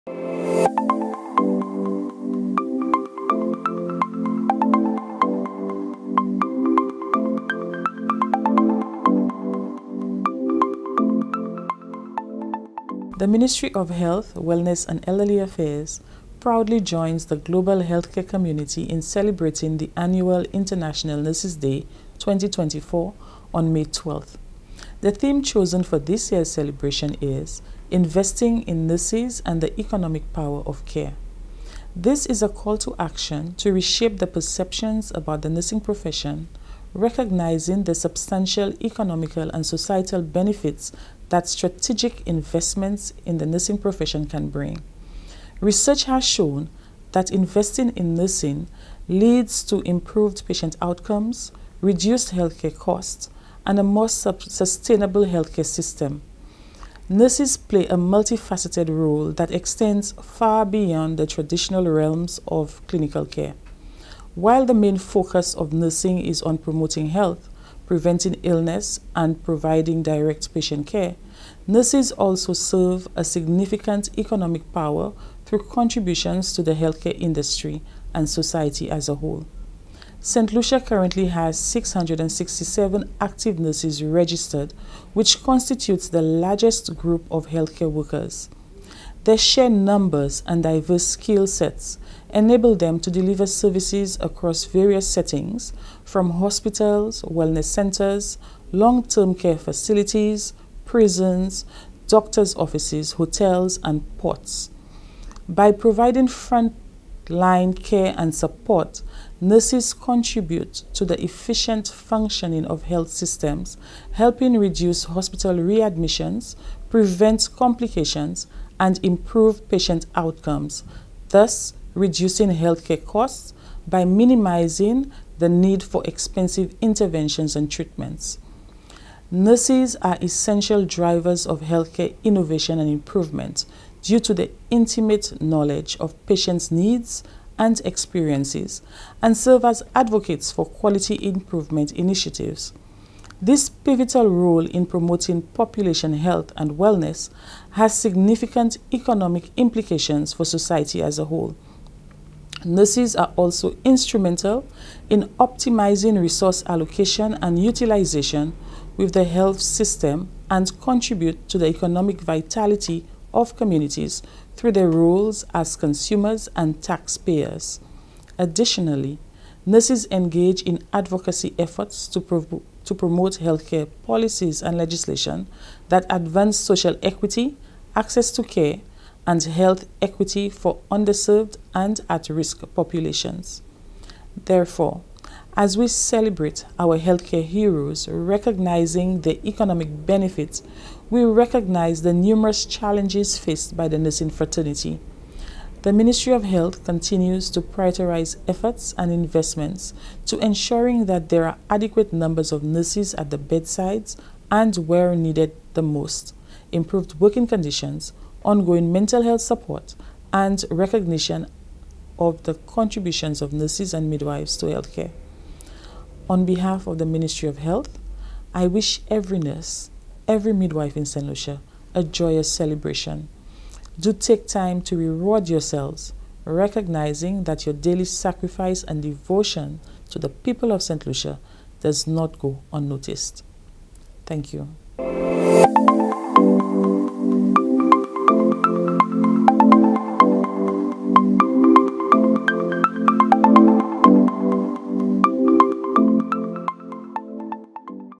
Ministry of Health celebrates International Nurses Day
The Ministry of Health joins the global healthcare community in celebrating the annual International Nurses Day, on May 12. The theme chosen for this year’s celebration is: Investing in Nurses and the Economic Power of Care.

This is a call to action to reshape perceptions about the nursing profession, recognizing the substantial economical and societal benefits that strategic investments in the nursing profession can bring.